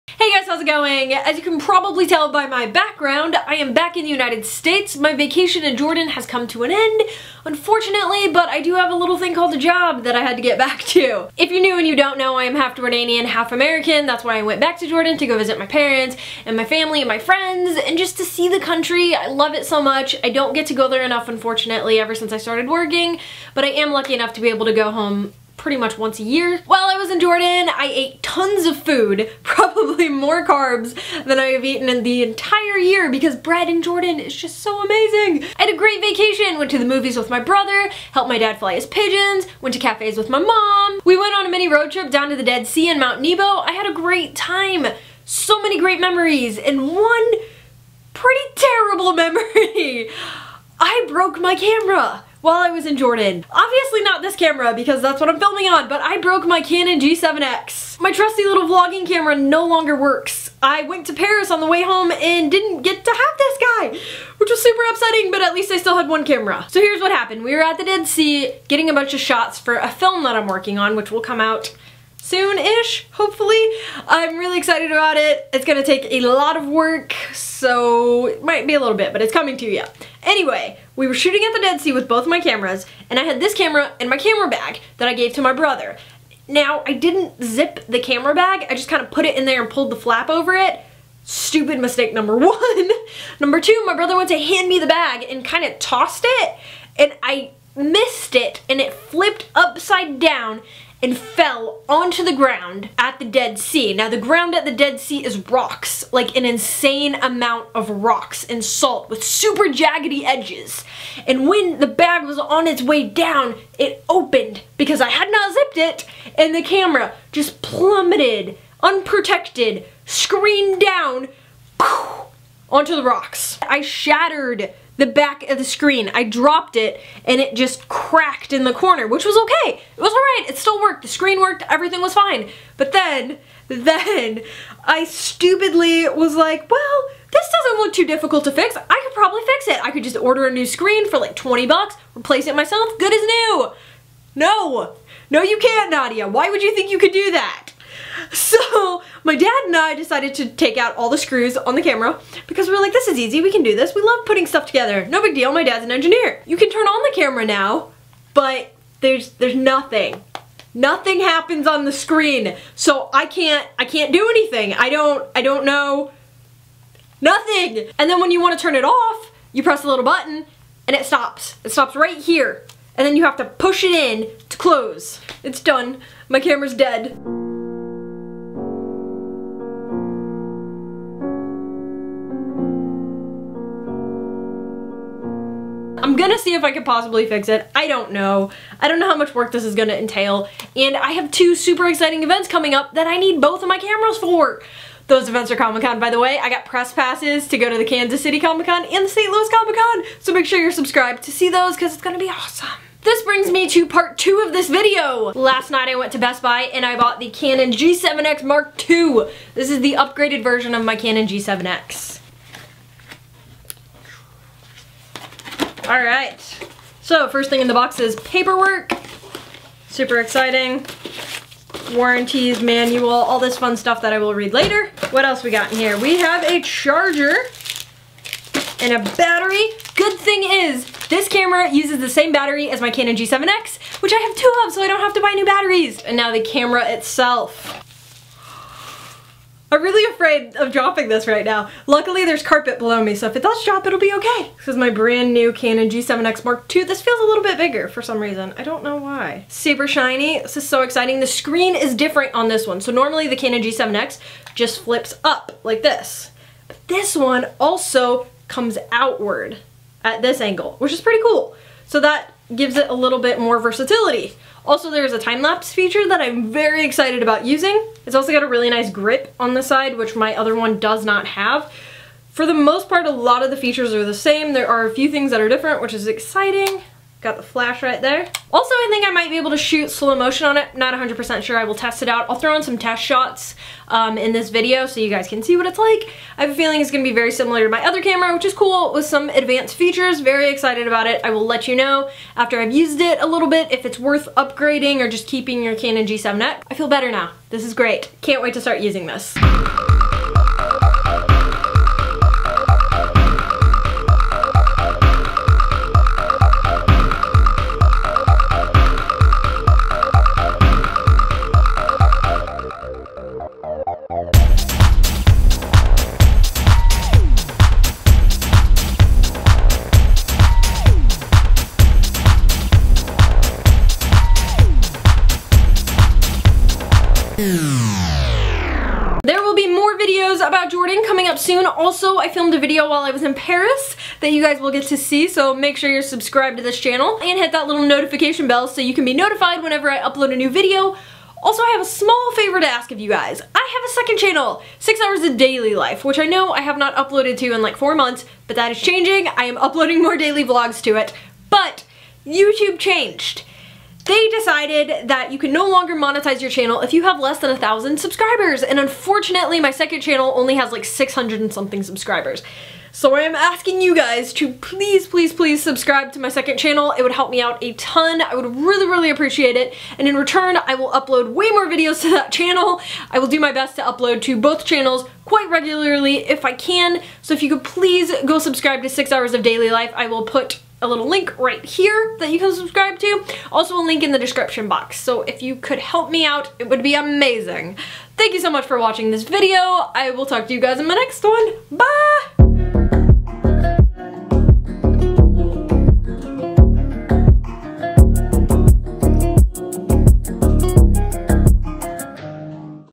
Broke My Camera At The Dead Sea!! (Canon G7X Mark II Unboxing)
Story of how I broke my camera at the Dead Sea while on vacation in Jordan. Also, unboxing of my new Canon G7X Mark II.

Storie: ghahknadia

Camera: Canon EOS 600D
Lens: Canon EFS 18-135mm
Microphone: Rode Rycote
Daily Vlog Camera: Canon G7X & iPhone 6
Drone: DJI Phantom 3 Standard